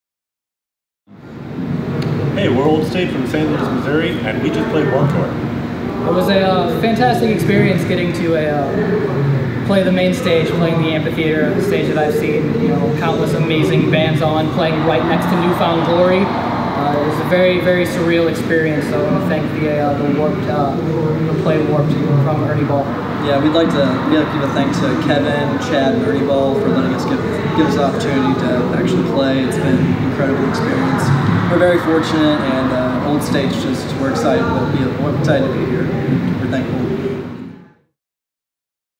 Ernie Ball: PLAY Warped Tour Day 29 Winners - Old State
Ernie Ball Play Warped Tour Day 29 winners Old State are one of more than 40 unsigned bands we're giving the chance to play the Warped Tour main stage.